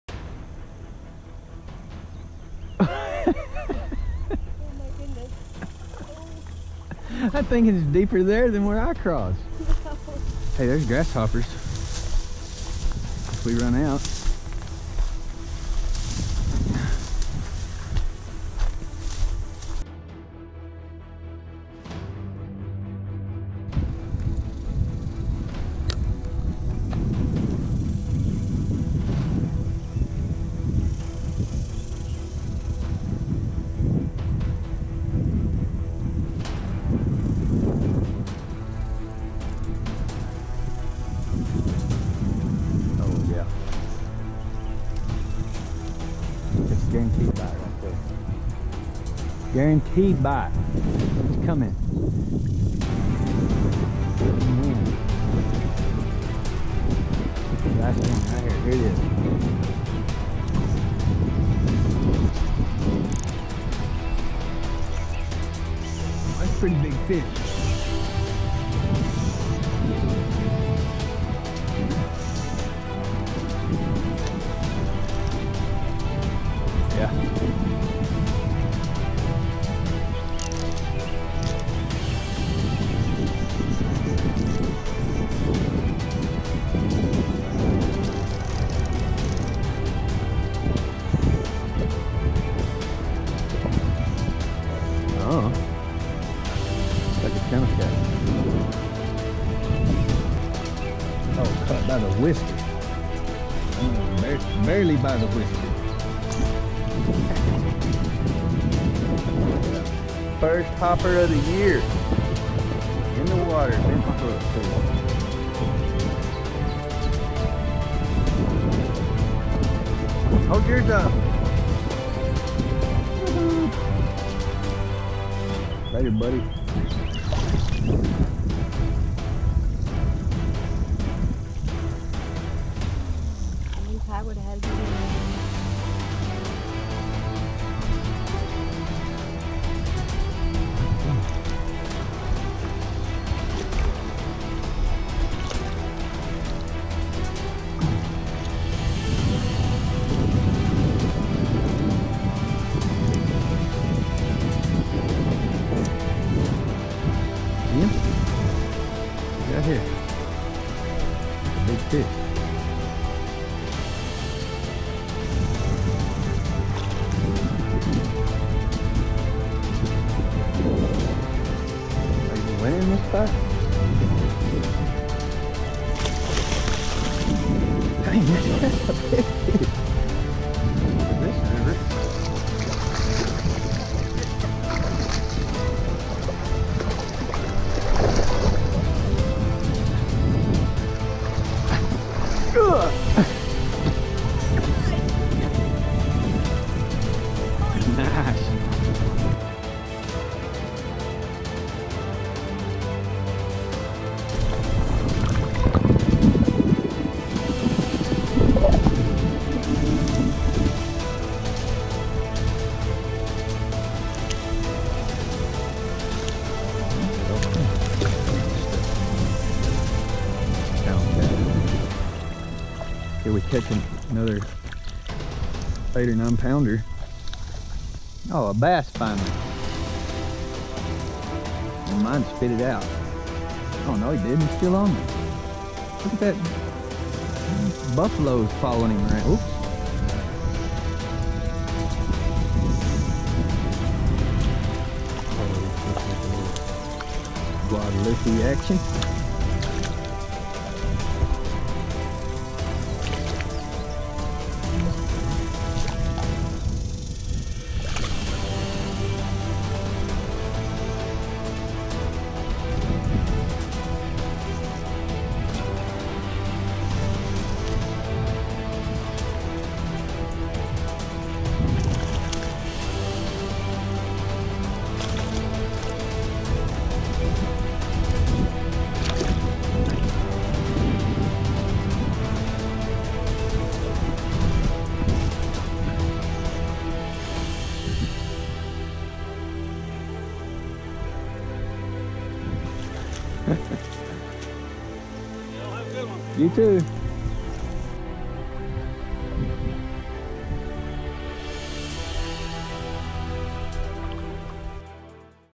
Hopper poppin’ the Brazos for channel cat
The best summertime fishing action in any river is on grasshoppers, every fish in the water eats them.